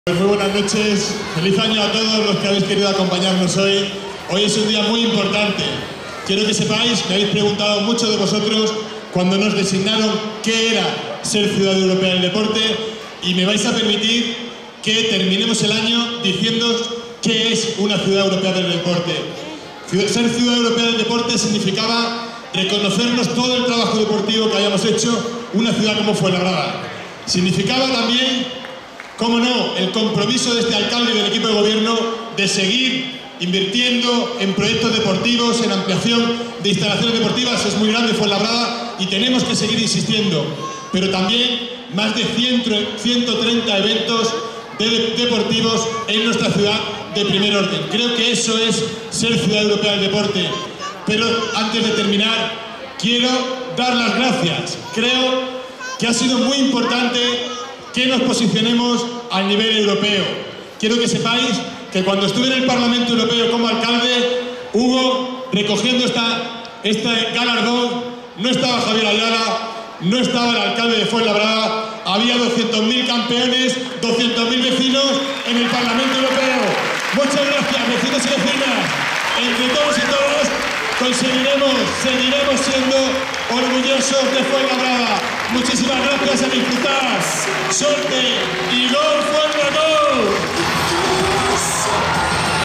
Intervención del alcalde en la Fiesta finalización ciudad europea deporte
Intervención del alcalde, Javier Ayala, en la Fiesta de despedida del año de Fuenlabrada como Ciudad Europea del Deporte